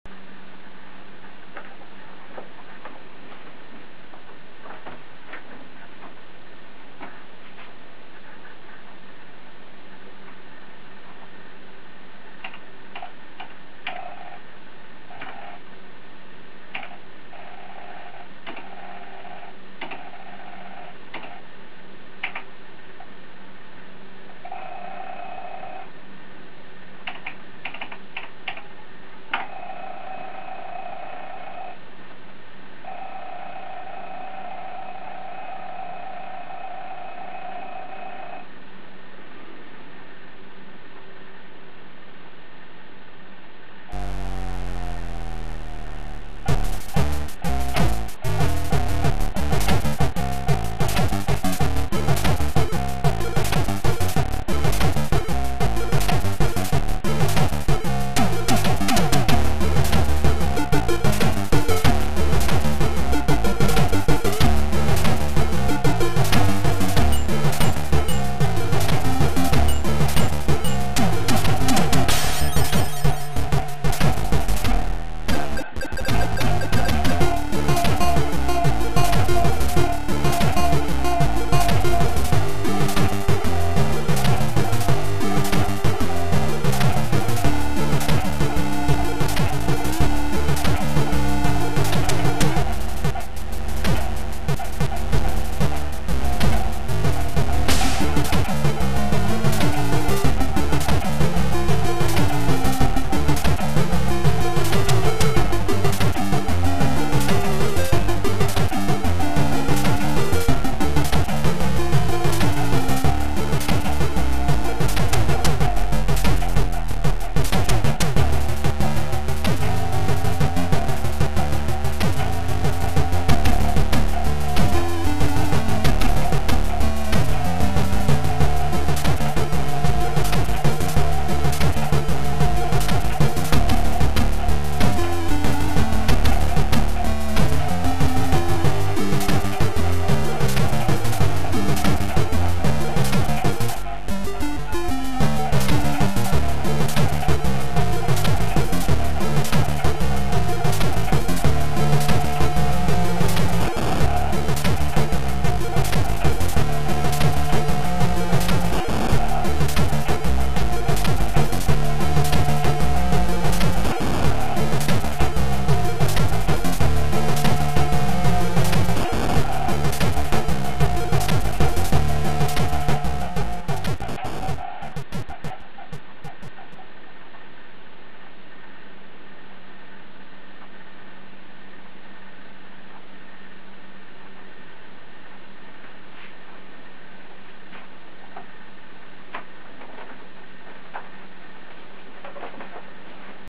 Atari XL Laptop runs "unplugged" by M.E.C.
Cool Demo by M.E.C. released at Glucholazy party in Poland. I boot it live on my XL Laptop. It is stored on the built-in CF-Drive.
The recorded music's volume was too little, so i overlayed it with a emulators output. They don't sync 100%, i try to get it perfect.